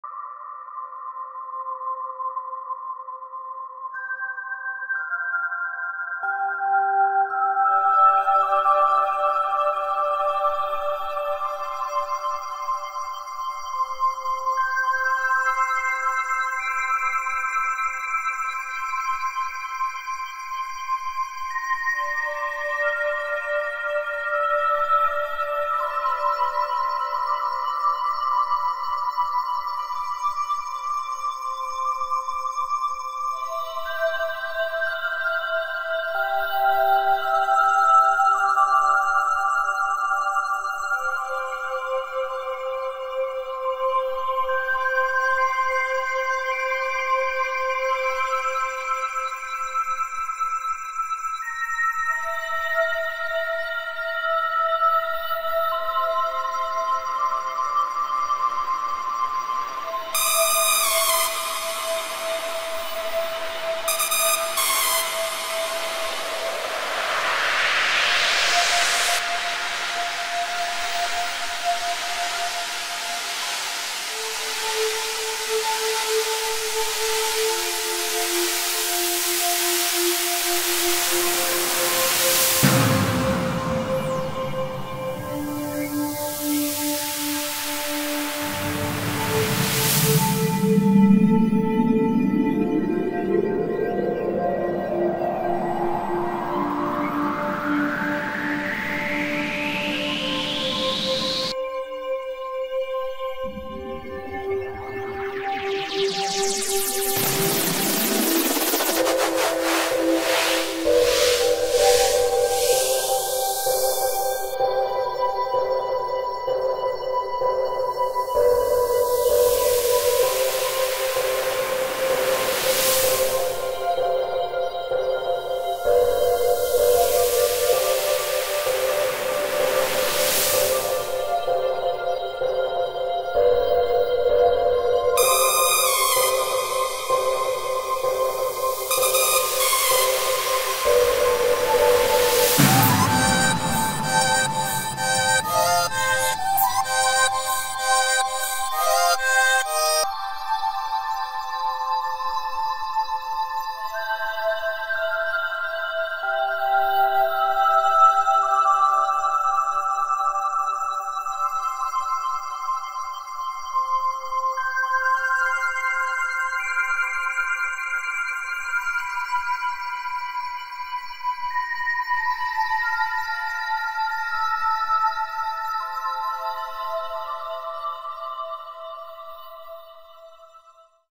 Invisible Cities: Terradieuse
invisiblecities A surreal trip beyond the confines of Italo Calvino's masterpiece, to discover more Cities: here is one that goes on a very long journey. @makeyourownworld